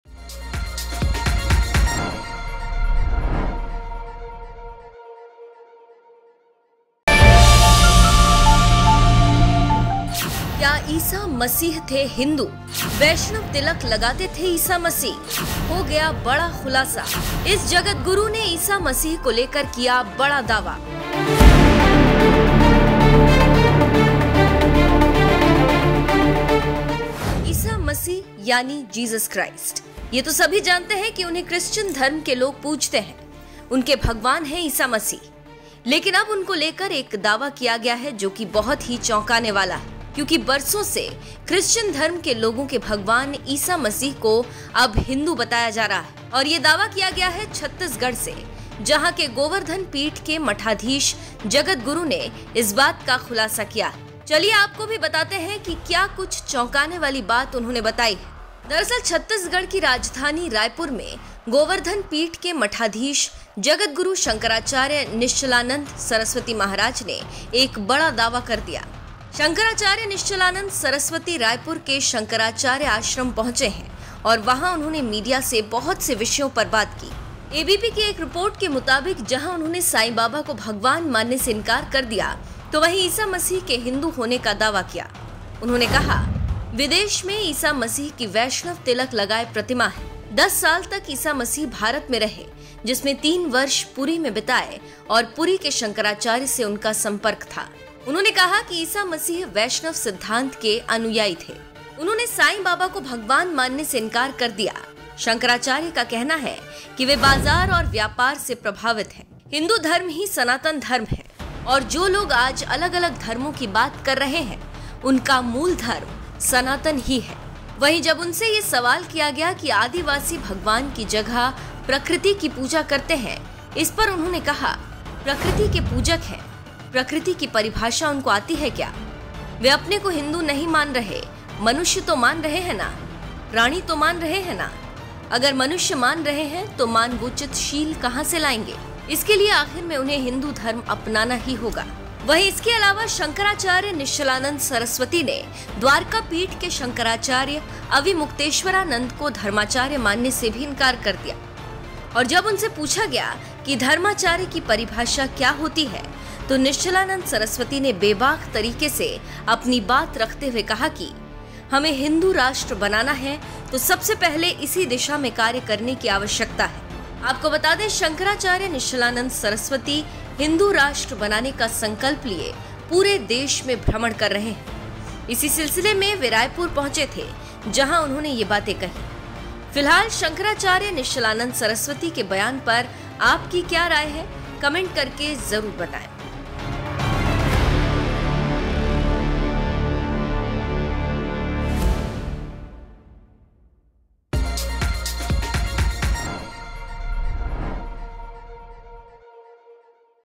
क्या ईसा मसीह थे हिन्दू ? लगाते थे वैष्णव तिलक, हो गया बड़ा खुलासा !
ईसा मसीह यानी jesus christ, ये तो सभी जानते है की उन्हें क्रिस्टन धर्म के लोग पूजते है, उनके भगवान है ईसा मसीह, लेकिन अब उनको लेकर एक दावा किया गया है, जो की बहुत ही चौकाने वाला है। क्युकी बरसो से क्रिस्टन धर्म के लोगो के भगवान ईसा मसीह को अब हिन्दू बताया जा रहा है। और ये दावा किया गया है छत्तीसगढ़ से, जहा के गोवर्धन पीठ के मठाधीश जगद्गुरु ने इस बात का खुलासा किया है।